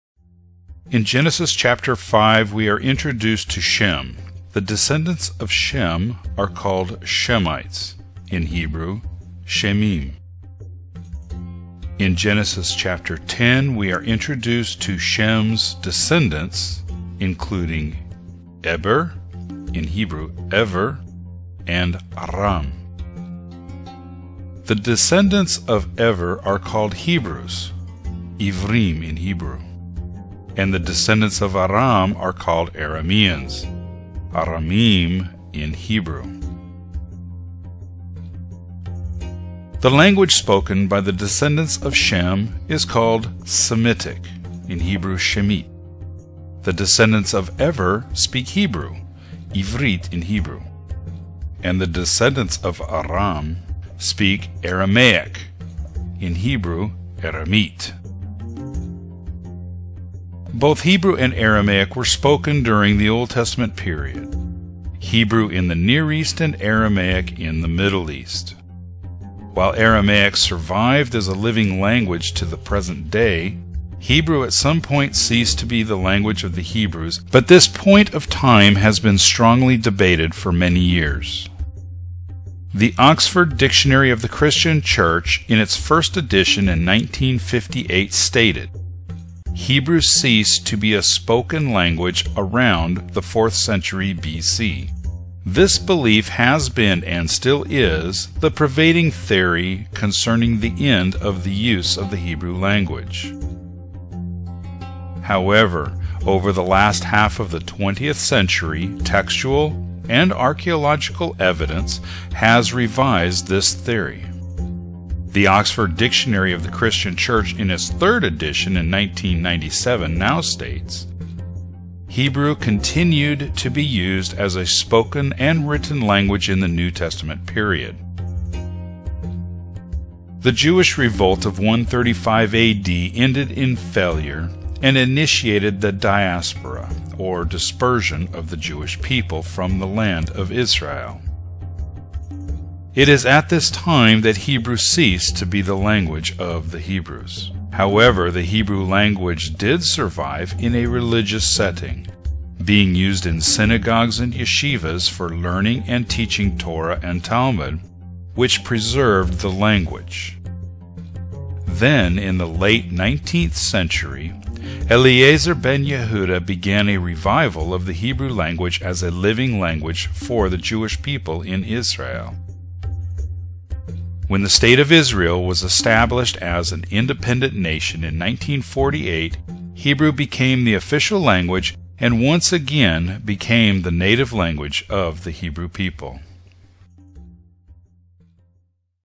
A History of Hebrew Part 15: The history of the language
A History of Hebrew DVD available through Amazon.Com

This is a segment of a much larger video production that I am working on and am looking for feedback (positive and negative) on the layout and content.

In Genesis chapter 5 we are introduced to Shem. The descendents of Shem are called Shemites (in Hebrew, Shemiym ). In Genesis chapter 10 we are introduced to Shem's descendents, including Eber ( in Hebrew, Ever) and Aram. The descendents of Ever are called Hebrews (in Hebrew, eevriym ) and the descendents of Aram are called Arameans (in Hebrew, Aramiym ).

The language spoken by the descendents of Shem is called Semitic (in Hebrew, shemiyt ). The descendents of the Ever speak Hebrew (in Hebrew,m eevriyt ) and the descendents of Aram speak Aramaic (in Hebrew, Aramiyt ).

Both Hebrew and Aramaic were spoken during the Old Testament period, Hebrew in the Near East and Aramaic in the Middle East.

While Aramaic survived as a living language to the present day, Hebrew at some point ceased to be the language of the Hebrews, but this point of time has been strongly debated for many years.

The Oxford Dictionary of the Christian Church, in it's first edition in 1958, stated; "[Hebrew]ceased to be a spoken language around the fourth century BC."

This belief has been, and still is, the pervading theory concerning the end of the use of the Hebrew language.

However, over the last half of the 20th Century, textual and archeological evidence has revised this theory.

The Oxford Dictionary of the Christian Church, in its third edition in 1997 now, states; "[Hebrew] continued to be used as a spoken and written language in the New Testament period".

The Jewish revolt of 135 A.D. ended in failure and initiated the Diaspora, or dispersion of the Jewish people from the land of Israel. It is at this time that Hebrew ceased to be the language of the Hebrews.

However, the Hebrew language did survive in a religious setting, being used in Synagogues and Yeshivas for learning and teaching Torah and Talmud, which preserved the language.

In the late 19th A.D. Eliezer Ben-Yehuda began a revival of the Hebrew language as a living language for the Jewish people in Israel.

When the state of Israel was established as an independent nation in 1948 Hebrew became the official language and once again became the native language of the Hebrew people.

Narration: Jeff A. Benner

Graphics: Jeff A. Benner

Photos: Paju